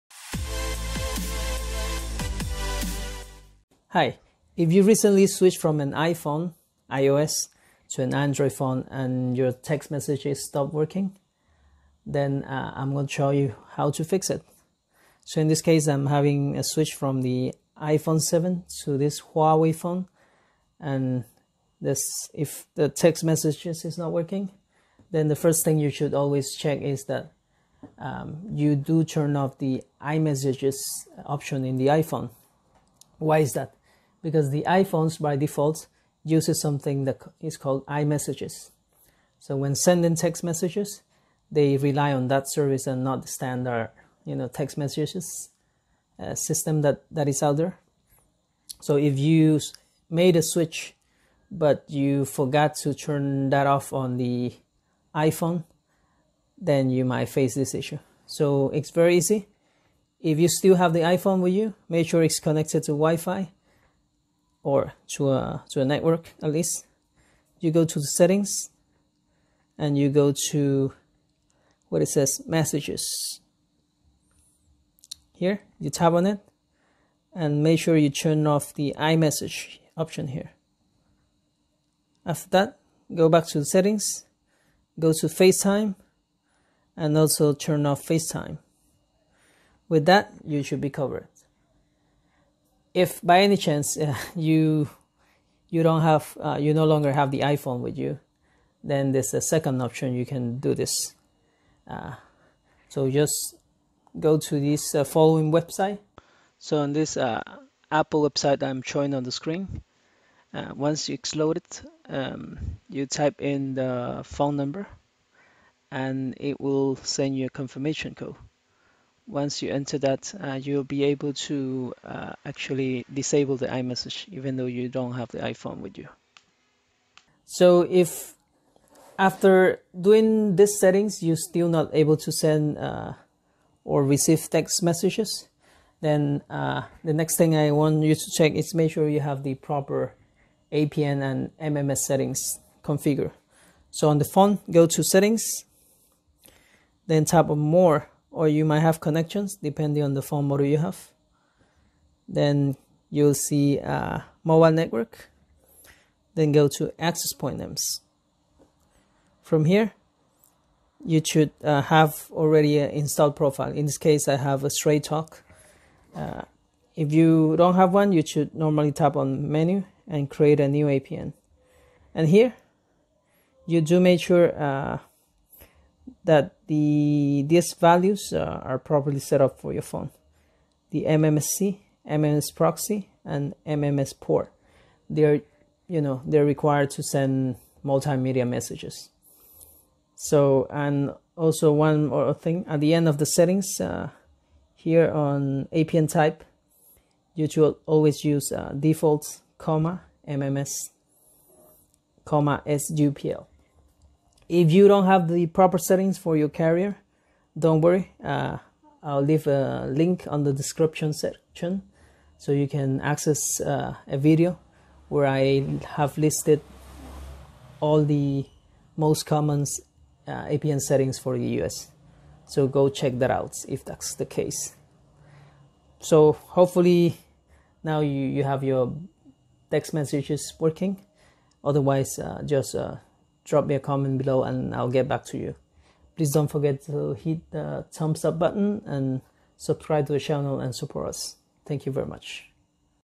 Android not Receiving Text after Switching from iPhone
If you switched to Android and are not getting texts from iPhone, here is the easy fix.

Before you switch from an iPhone iOS to an android or windows phone, there are a few things you should be aware of, one of those is turning off  or deactivating the iMessage option on your phone

iPhone uses the iMessage functionality to text between them, so if you have switched from iPhone to android but did not turned off the iMessage feature,  apple still thing you have the "iphone" with you and text messages coming out of another iphone to your number will be delivered through iMessage and not the standard txt messaging service

so it's very easy to solve this, just to to Settings, Messages and turn off iMessages.
fill in your number and confirm the code and you're done.  This is normally called the iMessage bug, as it shouldnt happen at all

if this doesnt solve it, then you'll need to check the APN and MMS settigns on the phone, as MMS or picture  messages rely on these settings, if they are not configured, then you wont be able to send or receive text messages

to check the mms settings you need for your carrer, please check this video